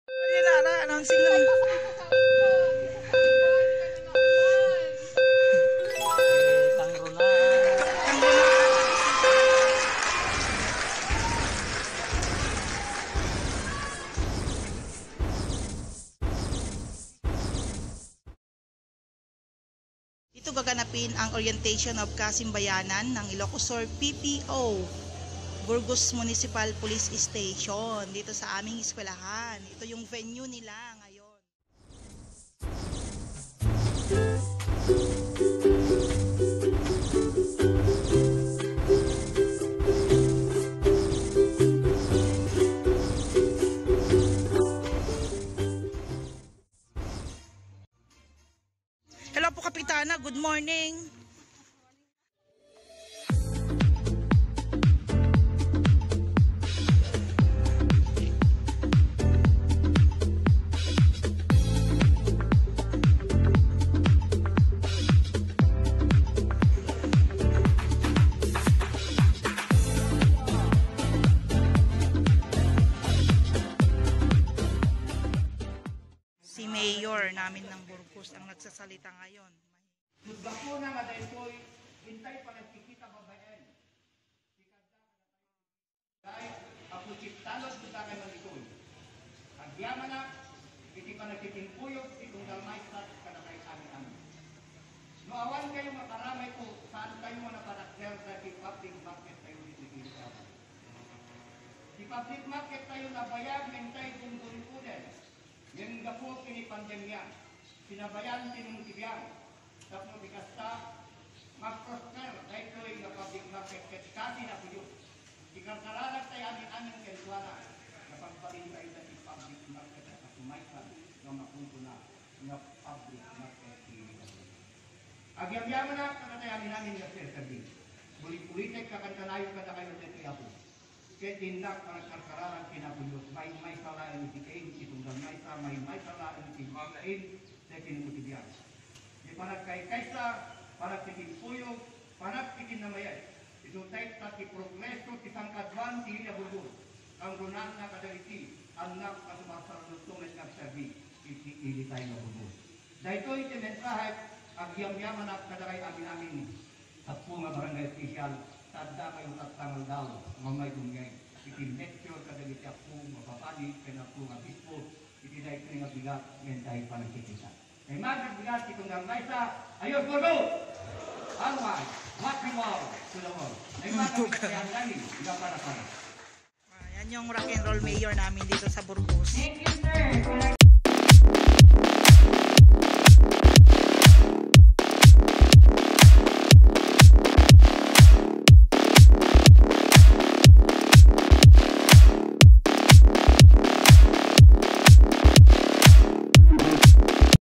The Orientation of KASIMBAYANAN | Kapulisan, Simbahan, Pamayanan
This was conceived to nridge tge gap between the community and the police  by bringing God and the Church.
Attended by Hon. Mayor Nathaniel Boy Escobar, the Police Force, Church Priest and Brgy. officials.